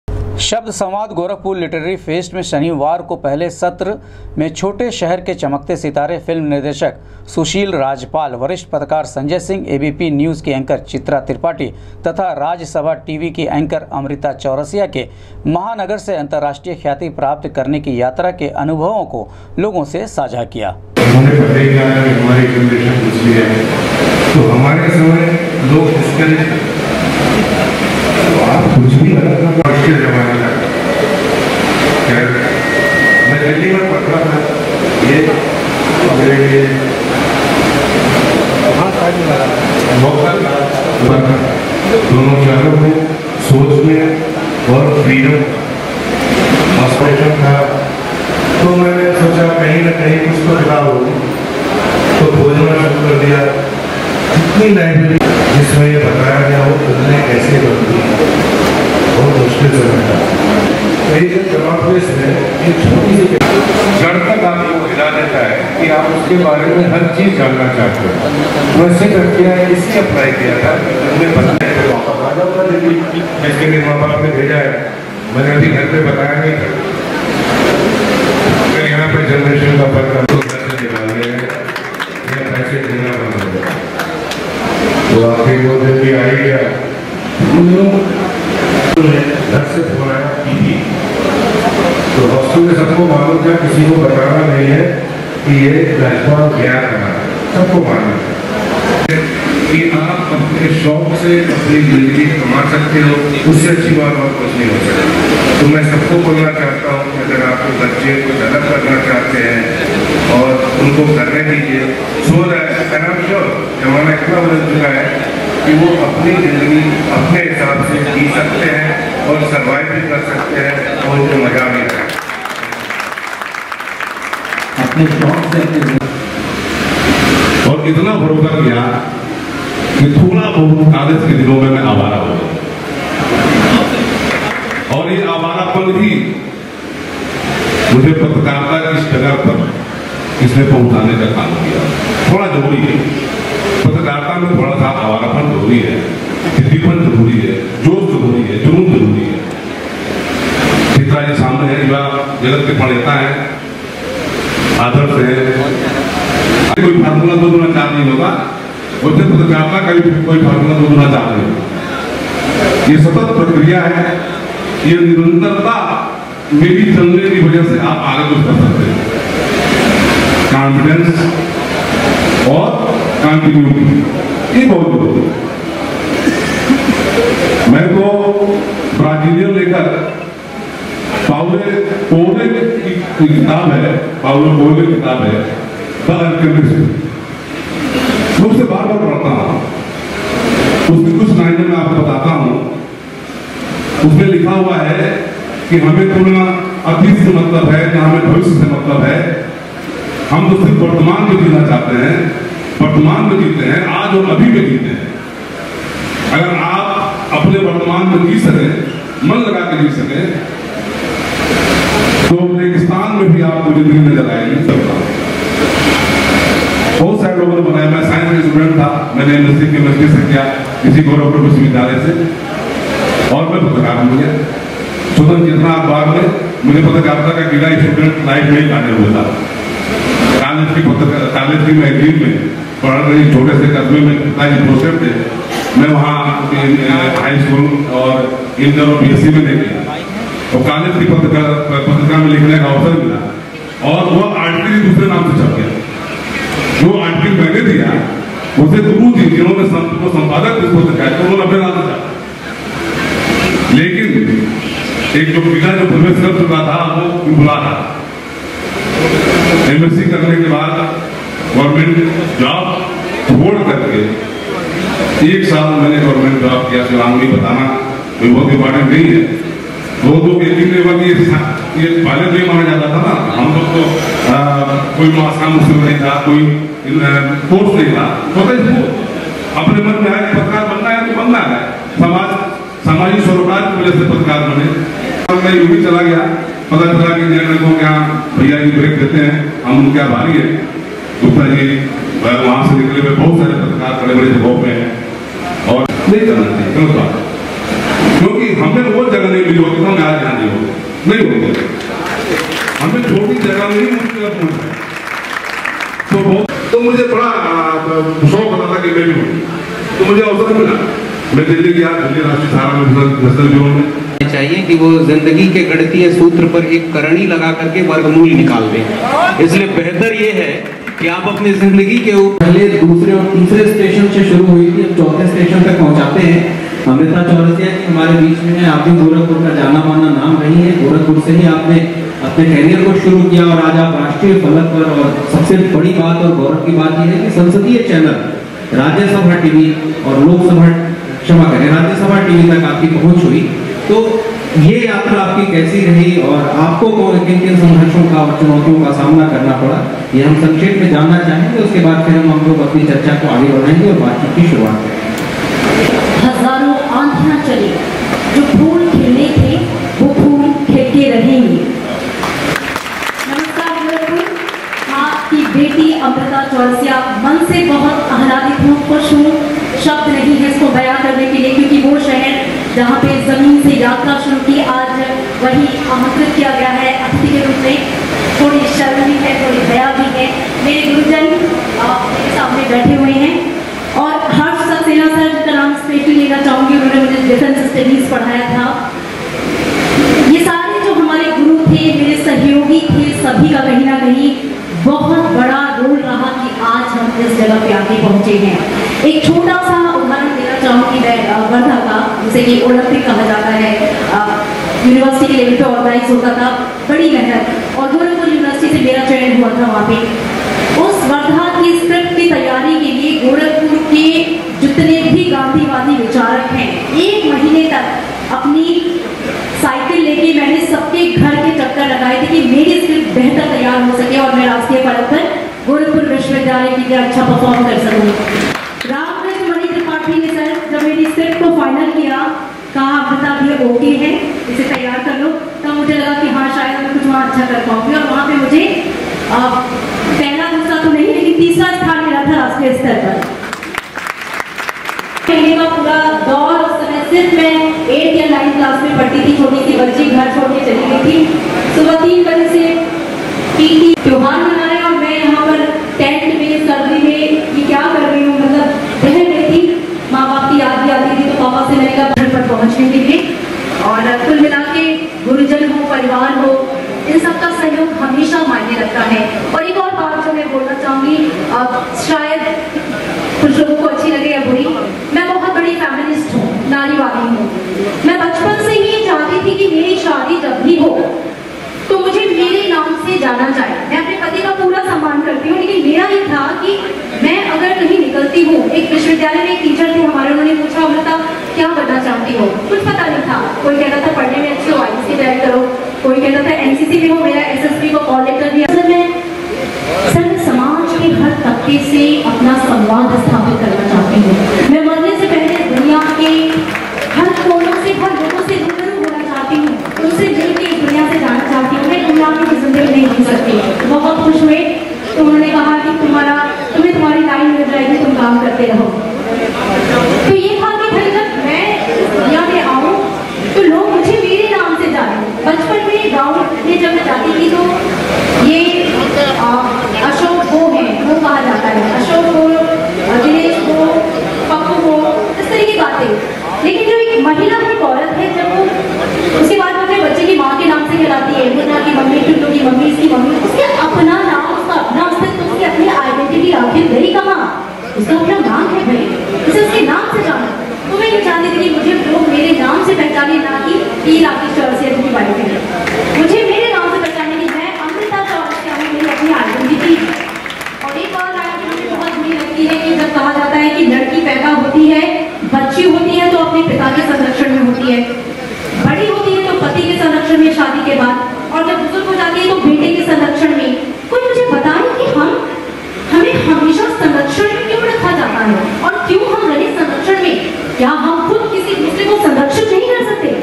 गोरखपुर लिटरेरी फेस्ट में ‘छोटे शहर के चमकते सितारों’ ने साझा किया, महानगर से अंतरराष्ट्रीय ख्याति
गोरखपुर लिटरेरी फेस्ट में ‘छोटे शहर के चमकते सितारों’ ने साझा किया, महानगर से अंतरराष्ट्रीय ख्याति प्राप्त करने का सफर